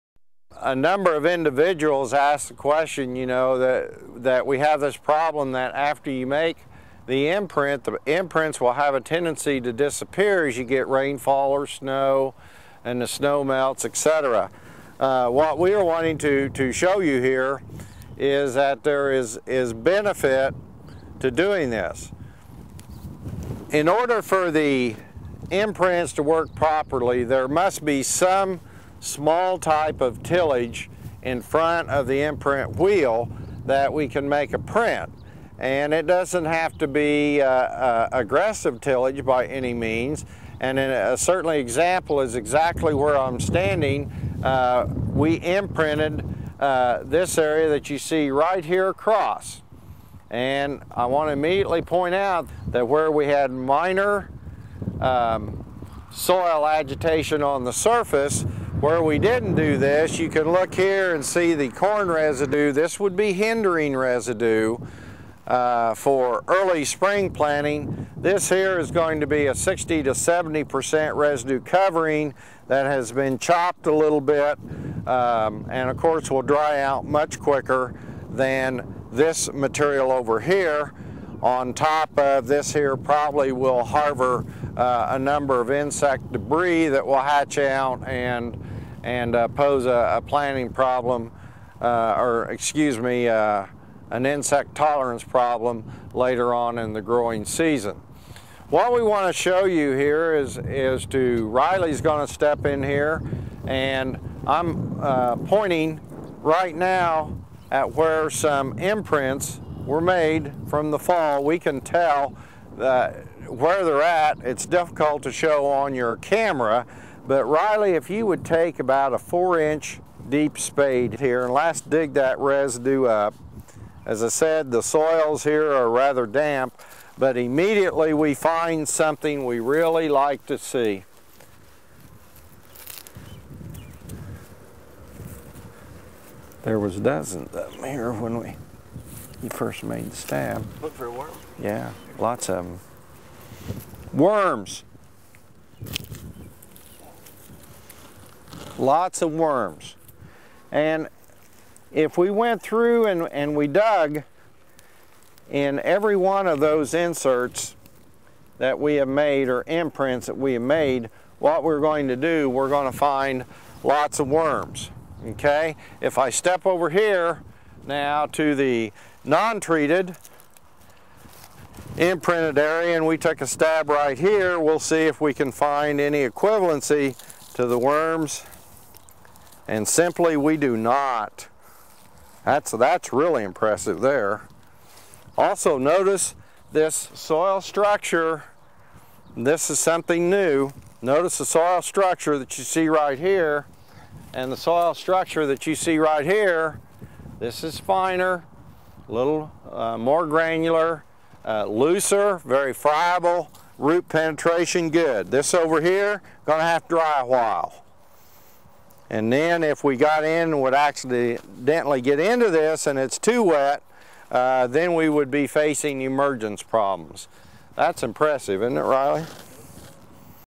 Soil Conditions
Dr. Roy Stephen, Arise Research, discusses the impact that imprinting has on soil conditions